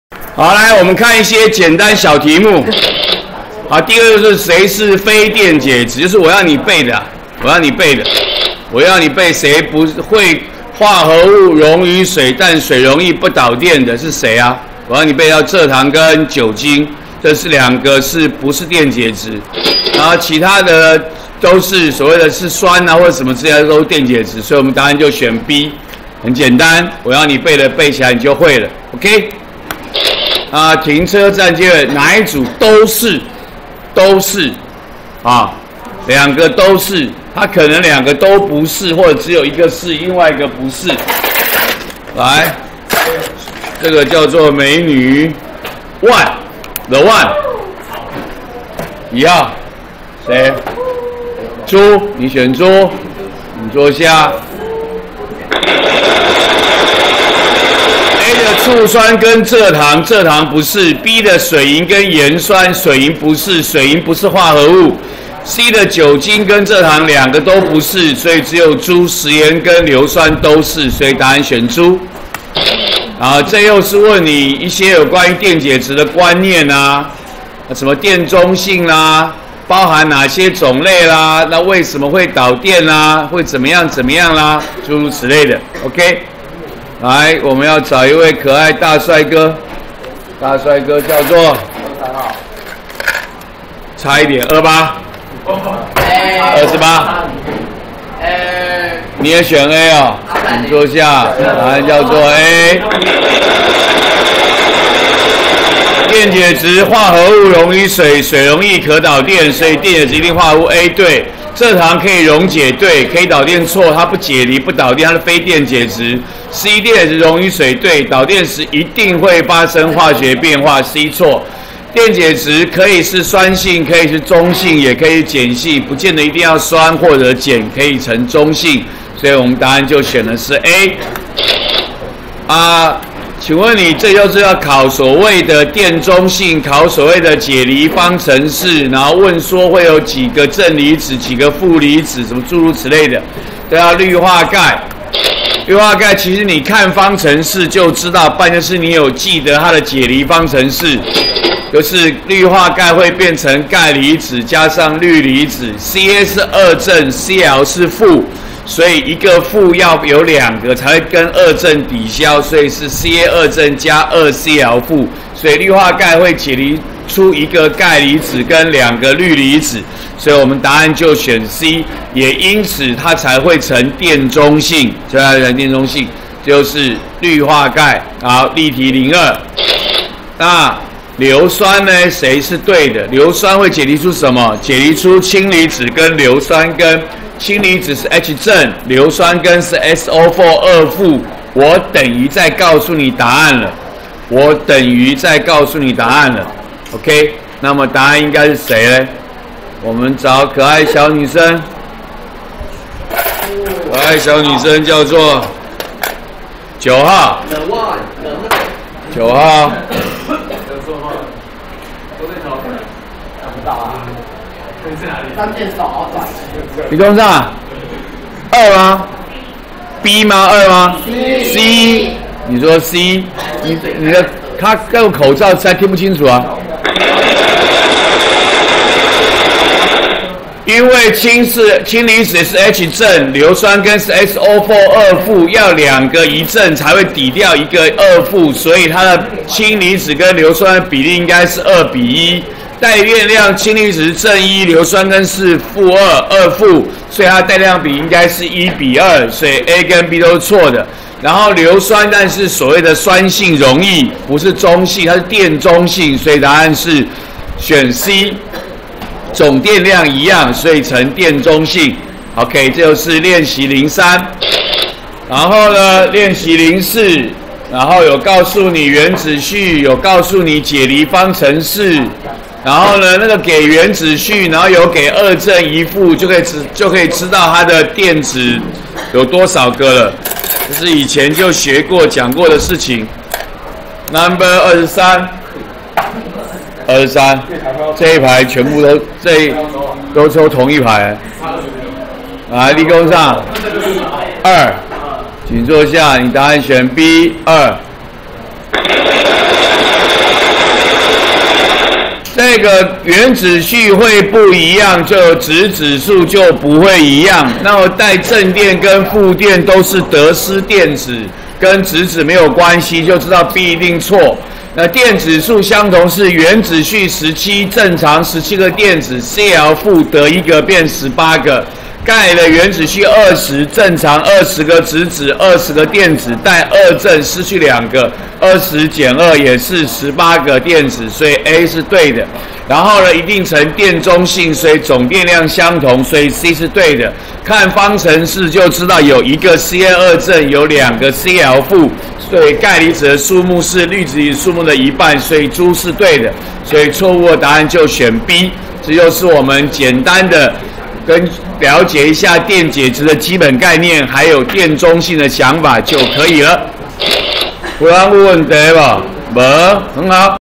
104學年複習酸鹼鹽02：例題01、02；練習01～04
例題01問哪個物質不是電解禹，練習01問哪一組物質都是電解質，問題02問關於電解質敘述，何者正確。例題02問氯化鈣解離，正負離子數目，練習03問硫酸解離的敘述，何者正確，練習04給氯、鈣原子序，及氯化鈣解離方程式，問離子數、電子數、質子數、總電量關係何者錯誤。